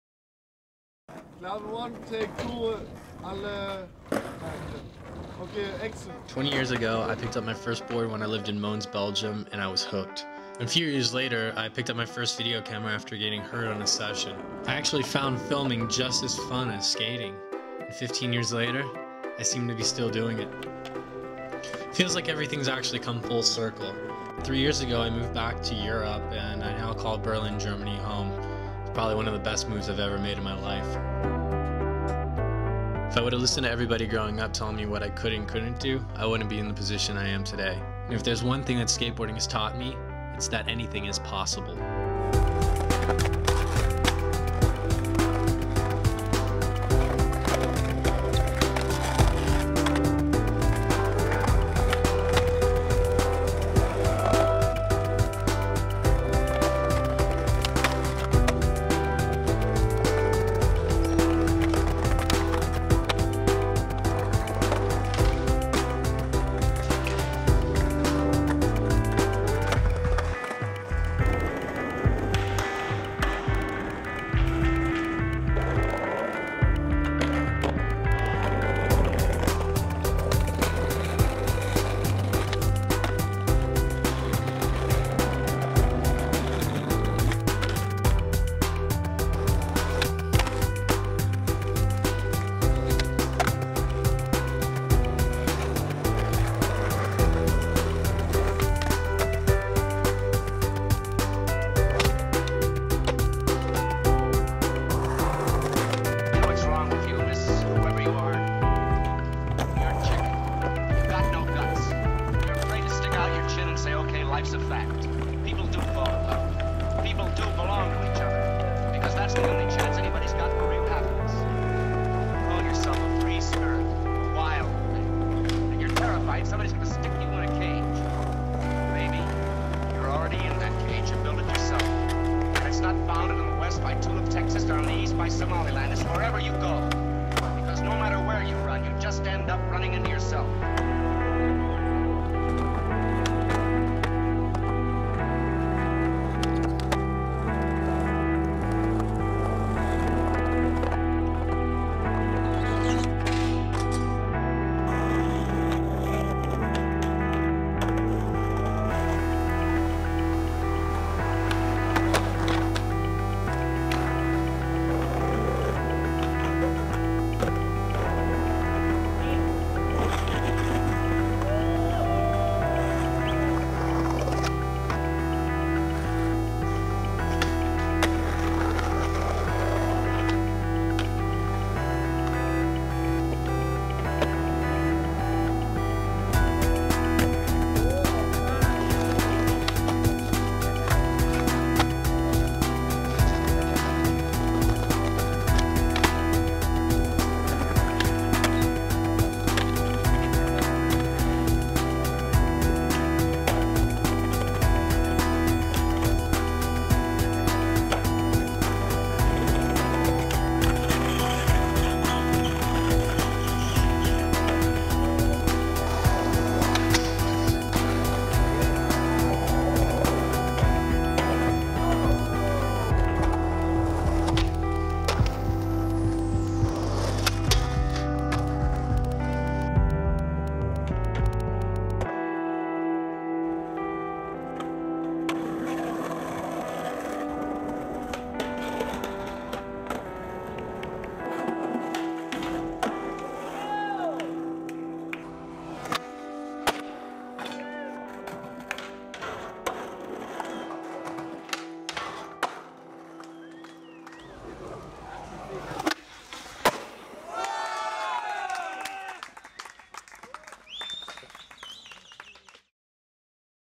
Puzzle Video 'Filmed By' Segment
This is my Puzzle 'Filmed By' segment I did for the March/ April 2009 video.